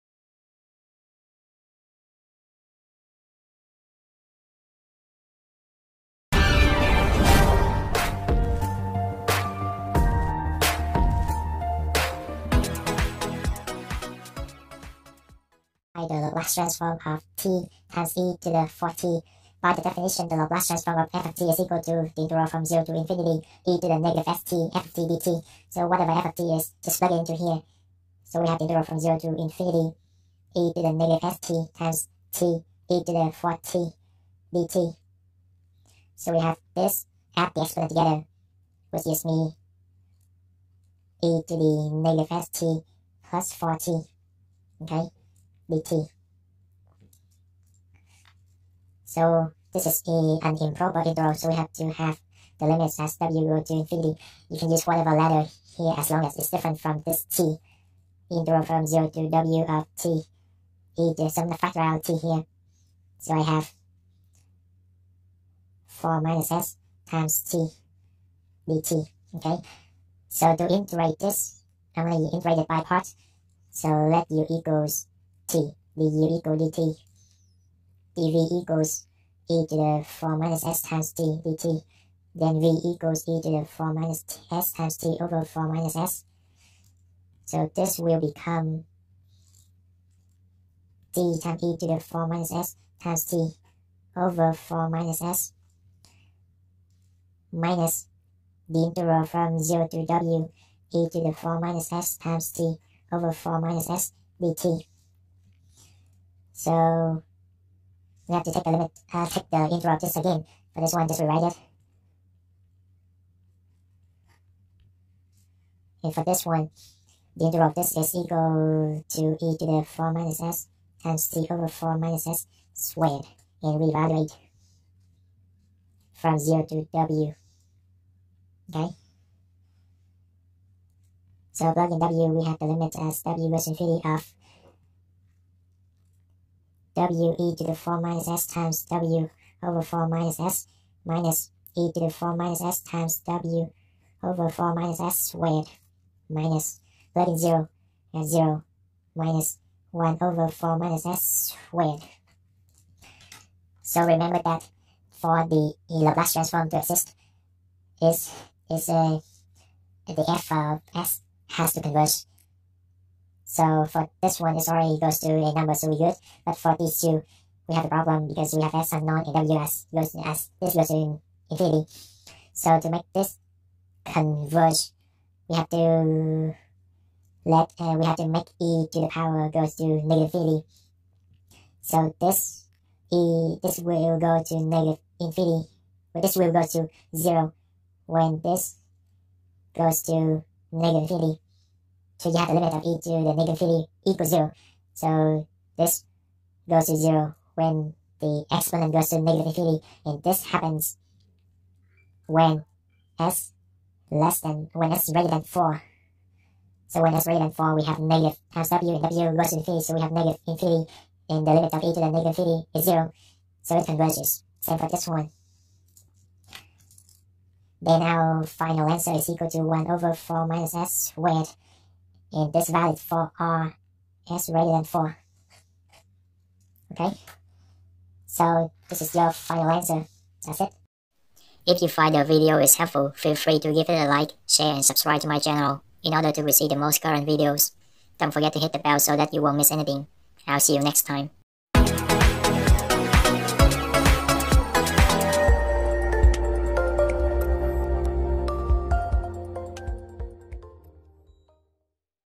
7.1-13 Find the Laplace Transform of te^4 | DE
Find the Laplace Transform of this function using its definition

f(t) = te^4

FYI: Access the table of Laplace Transform of some basic functions